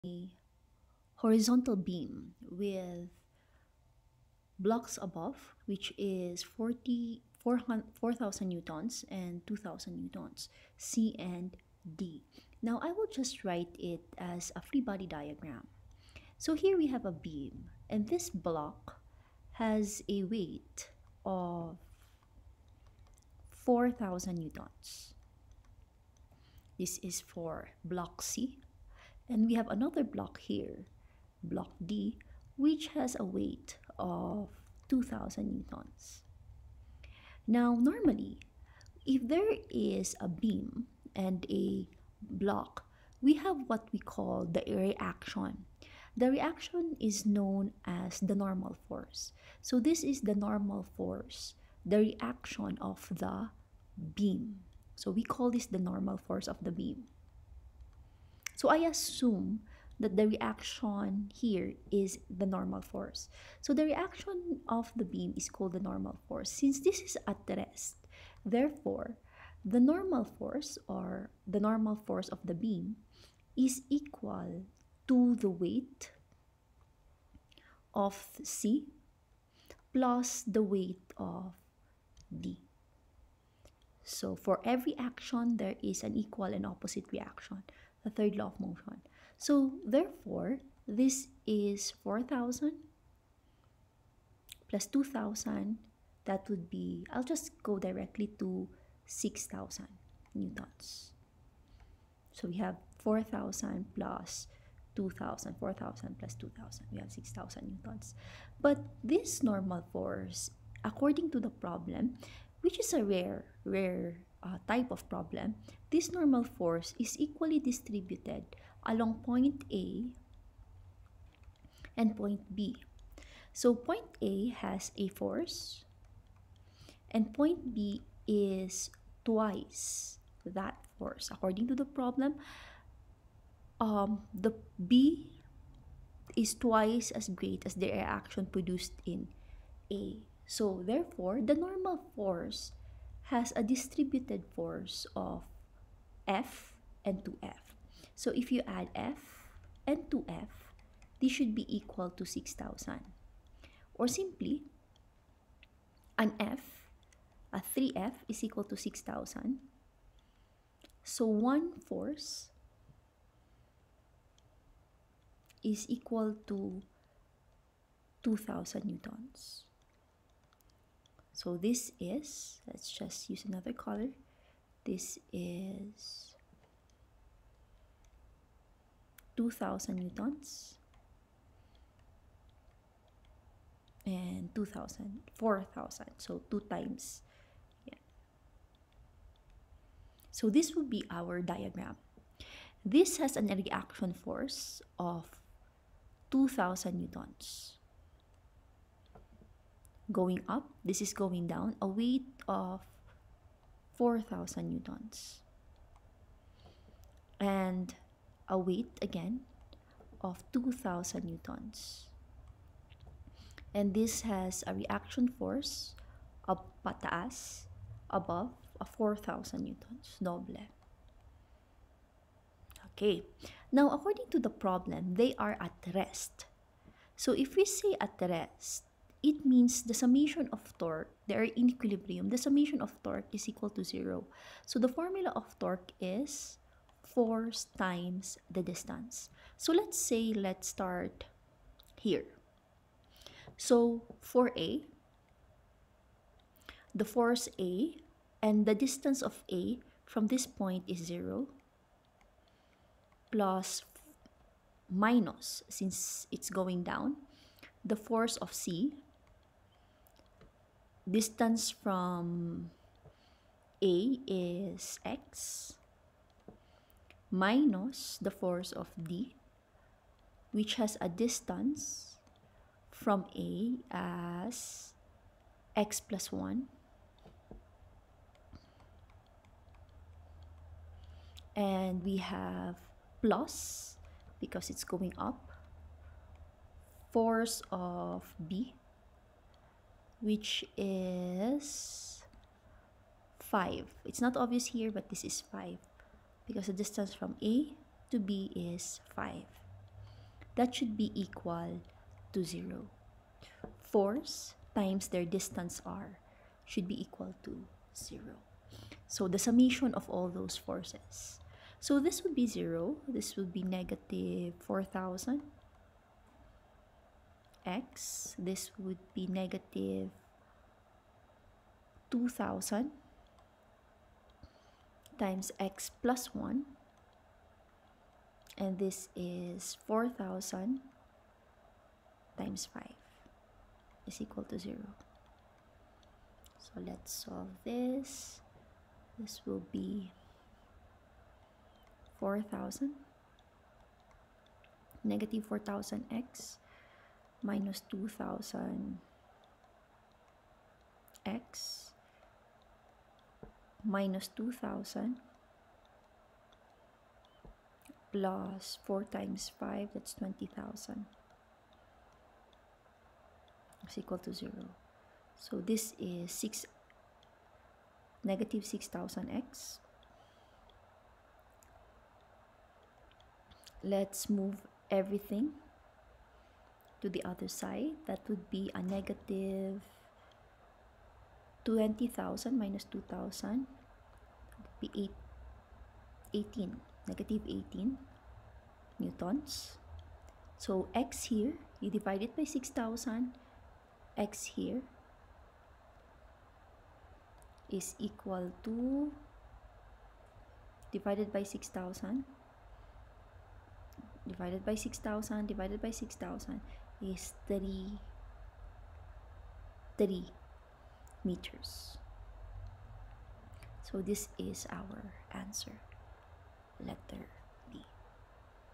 Physics 7 (2014): Blocks C and D, weighing 4000N and 2000N, rest on a horizontal beam AB.
Physic 2014 version:
7. Blocks C and D, weighing 4,000 newtons and 2,000 newtons, respectively, rest on a horizontal beam AB, as shown above. If the reaction produced at B is twice as great as the reaction produced at A, how far from A will the weight of Block C act?